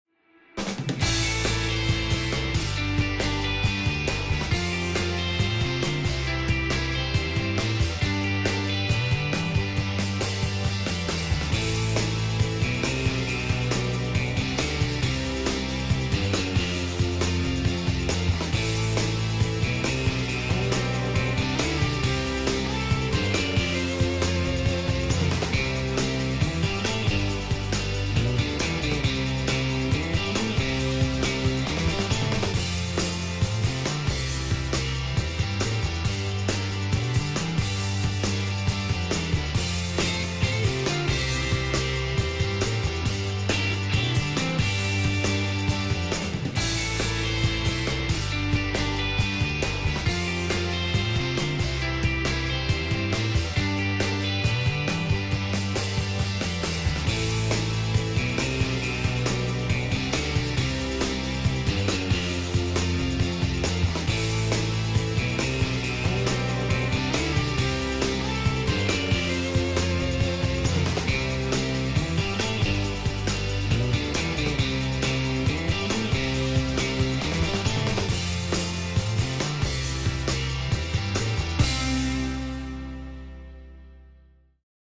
Prochem Ink Solv
Watch Ink Solv easily remove pink lipstick from carpet